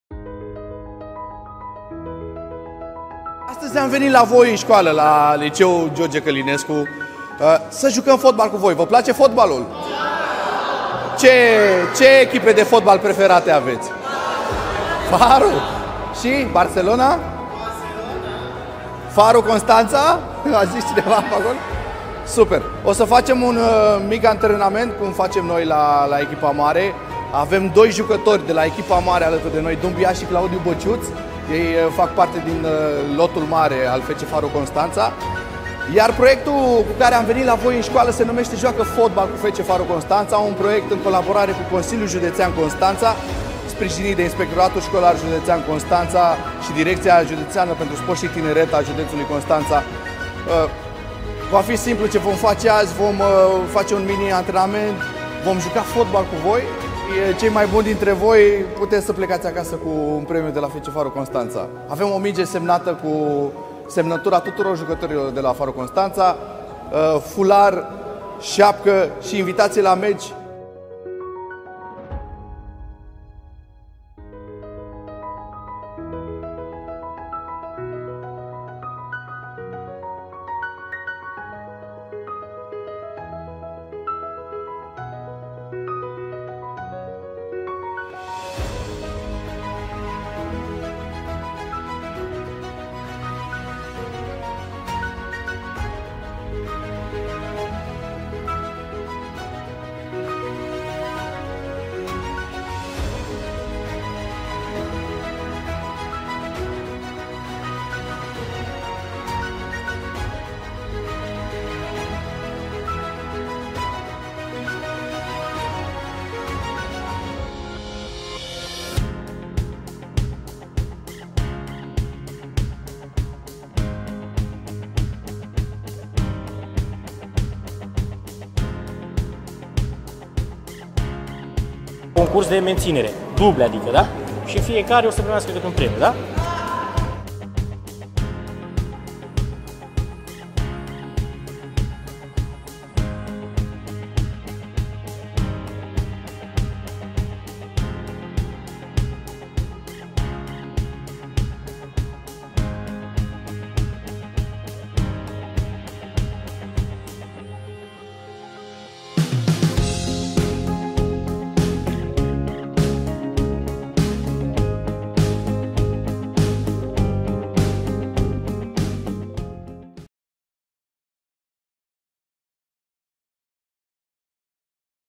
„Joacă fotbal cu FC Farul Constanța”, la Liceul Teoretic George Călinescu Constanța
Juniorii de la Academia Farul, pregătiți de antrenorul Claudiu Moraru, alături de doi jucători de la echipa de seniori a FC Farul, Madikaba Doumbia și Claudiu Băciuț, au poposit la Liceul George Călinescu Constanța, în cadrul Proiectului „Joacă fotbal cu FC Farul Constanța”, realizat cu sprijinul Consiliului Județean Constanța și în parteneriat cu Inspectoratul Școlar Județean Constanța și Direcția pentru Sport și Tineret a Județului Constanța, care are drept obiectiv promovarea mișcării ca stil de viață sănătos și descoperirea de noi talente pentru fotbalul constănțean.

Primiți cu entuziasm, alb-albaștrii le-au arătat elevilor cum se desfășoară o ședință de pregătire și i-au invitat să li se alăture la antrenament. Iar propunerea a fost primită cu multă bucurie și interes, cei mici efectuând cu multă seriozitate exercițiile fizice.

La fel a fost primită și invitația la un joc amical între echipa liceului și cea a alb-albaștrilor, desfășurat în sala de sport. La finalul evenimentului, elevii și-au testat talentul la concursurile de îndemânare, cei mai buni fiind remarcați de antrenorul Dorel Zaharia și răplătiți cu premii constând în echipament sportiv cu însemnele FC Farul, mingi cu semnăturile jucătorilor noștri, șepci, rucsacuri și tricouri ale echipei noastre, ediția limitată de Centenar care marchează 100 de ani de fotbal la Constanța.